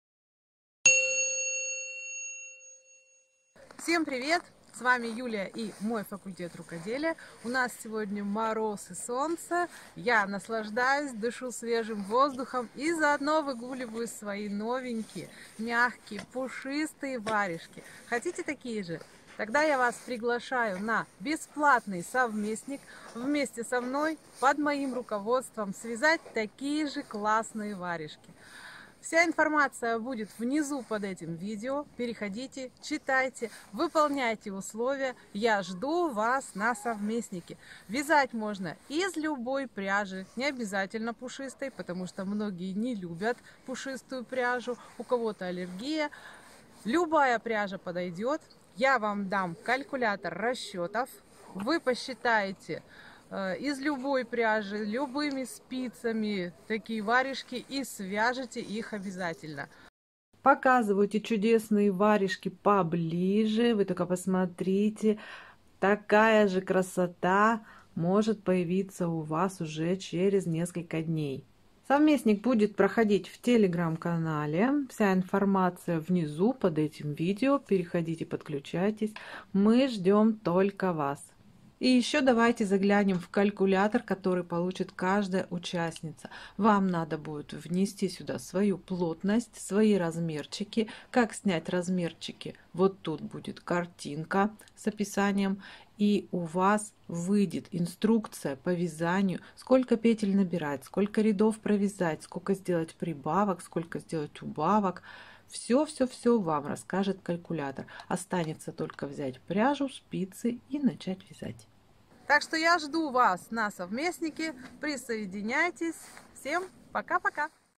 БЕСПЛАТНО! Вяжем "Пушистые варежки" из Пуха норки: с Калькулятором расчетов
Каждая участница получит от меня КАЛЬКУЛЯТОР расчетов.

Рада буду видеть Вас на моем совместнике по вязанию варежек✨
В этот раз мы будем вязать пушистые, мягкие и очень тёплые варежки! Ведь скоро Новый год, а они могут стать отличным подарком Вашим близким.

В рамках совместника Вас ждёт:
👉🏼 КАЛЬКУЛЯТОР расчетов, который моментально сделает расчёт для любых варежек☺️
👉🏼 telegram-канал, где будет собрана вся информация
👉🏼 подробные видео-уроки с объяснениями
👉🏼 возможность задавать мне вопросы
👉🏼 а также бессрочный доступ ко всем материалам совместника❗️

Рекомендации по пряже я дам обязательно, но Вы можете взять свою - и Вам не придется попадать в плотность! КАЛЬКУЛЯТОР поможет сделать расчёты для любой пряжи и для любого размера спиц!

И когда я говорю, что Вам не придётся ничего считать - я говорю правду!

КАЛЬКУЛЯТОР расчетов поможет Вам связать любые варежки:
▶️ однотонные, лицевой гладью
▶️ с любым узором и даже жаккардом
▶️ на любой размер рук (детские, женские, мужские - какие пожелаете❗️)
▶️ с резинкой или без
👉🏼 Вы научитесь моделировать варежки своей мечты🥰

Вам останется только взять спицы, пряжу, снять мерки и начать вязать 🧶

Мы остановимся с Вами на базовой модели варежек, поэтому ГАРАНТИРУЮ - у Вас все получится!

Для того, чтобы принять участие, Вам необходимо выполнить всего 3 условия:
▶️ в комментариях под этим видео написать: Участвую!

Совместник будет проходить в Telegram-канале, поэтому сейчас Вам необходимо:
👉🏼 и следить за новыми публикациями на нем

И помните… Все, что сделано руками, сделано от сердца🧡 До встречи на совместнике!

СПРАВА ВНИЗУ ЭКРАНА ЕСТЬ КНОПКА "СУБТИТРЫ" - кому мешают - просто отключайте.